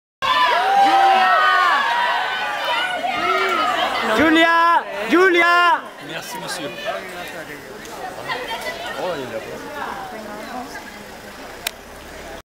Richard Jenkins - Zinemaldia 2010 (02)
El gran actor Richard Jenkins firma autógrafos en el Zinemaldia 2010, Festival Internacional de Cine de San Sebastián.